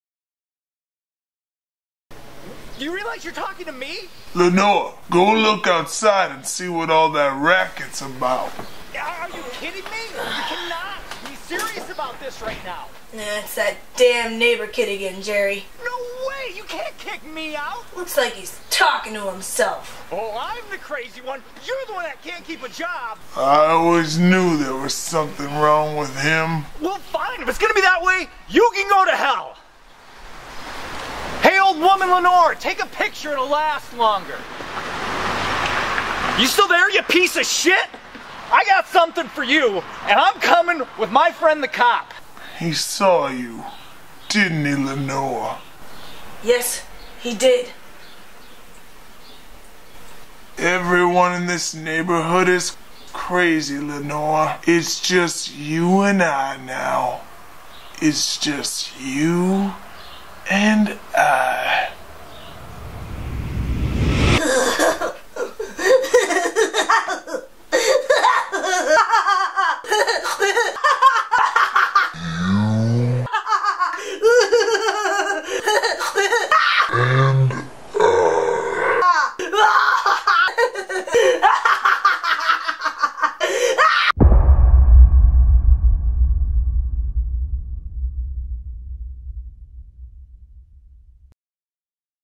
Off Their Meds
The 3rd Kind's intro sketch for "Stunt Double", one of the categories of Northern Spark's 10 Second Film Festival. Minneapolis 2012.

Lenore - Hannah Wydeven
Neighbor Kid - Travis Carpenter

All videos shot & edited by Dave Christians and/or Kevin Albertson.